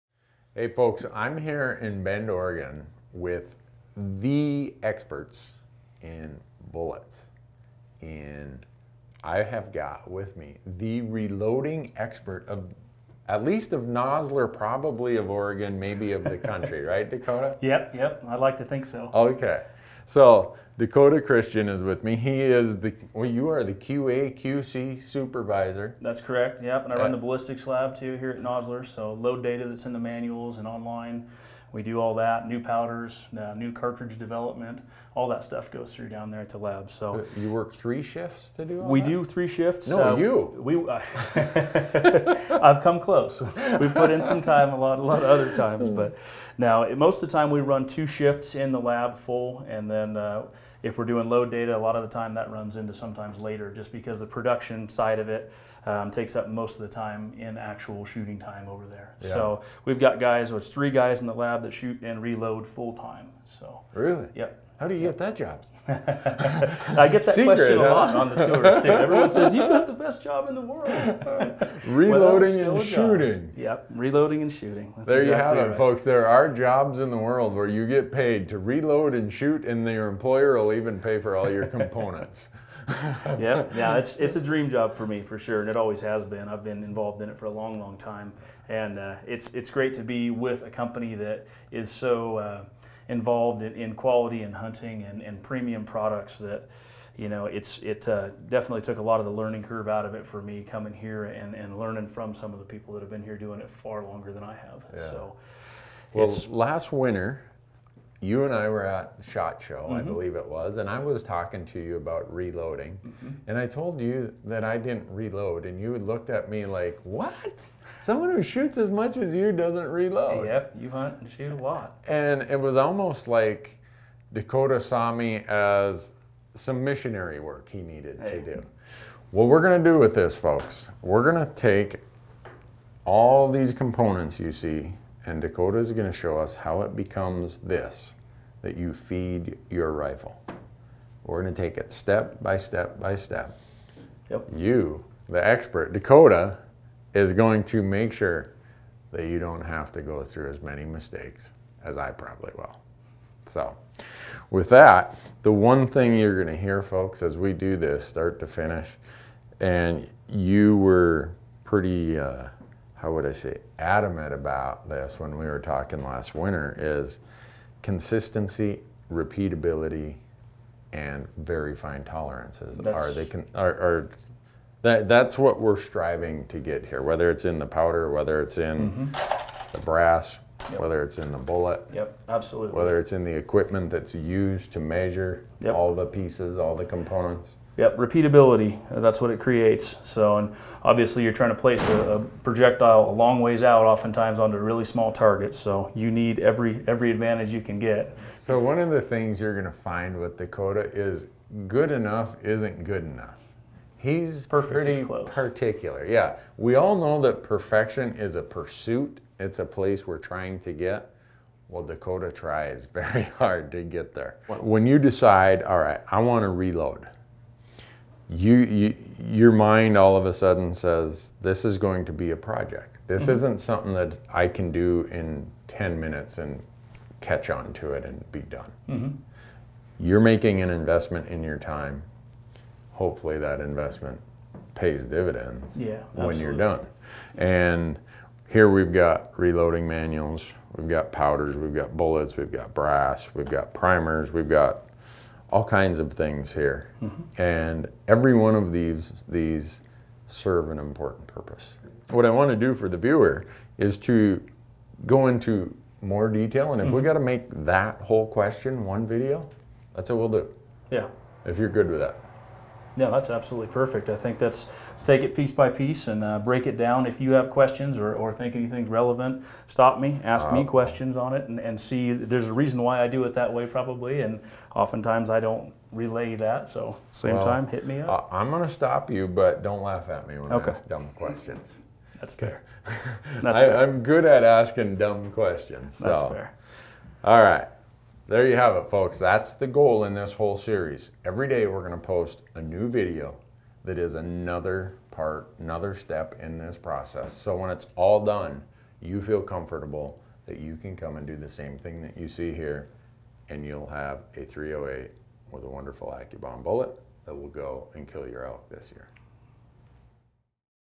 Nosler Teaches Randy Newberg How to Reload Ammunition (Part 1)
Randy Newberg, Hunter has never been a reloader, despite shooting thousands of rounds of ammunition.  Randy has traveled to Nosler Ammunition in Bend, Oregon where the Nosler experts will teach him the fine details of reloading your own ammunition.

This first video is an overview.  Each video in this series goes into depth on each topic important to reloading, including; bullet selection, powder selection and features of each powder, seating depth, primers, tweaking the load to match your rifle, and ends with a Q&A session.